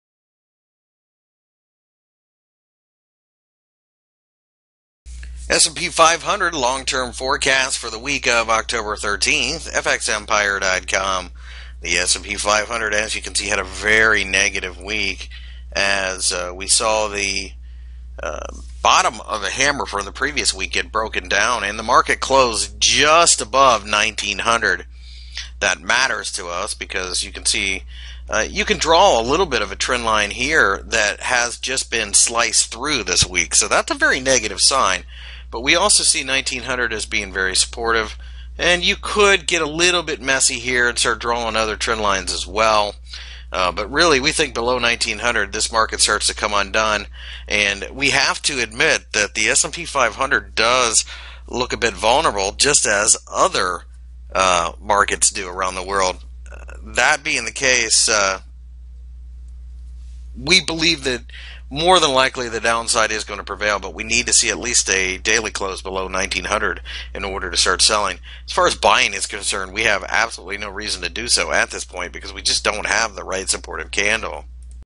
S&P 500 Index forecast for the week of October 13, 2014, Technical Analysis
- October 13, 2014 index weekly technical analysis for the S&P 500 INDEX.Find more information about Forex and Commodities News Technical Analysis Fundamental Analysis and Brokers on FX Empire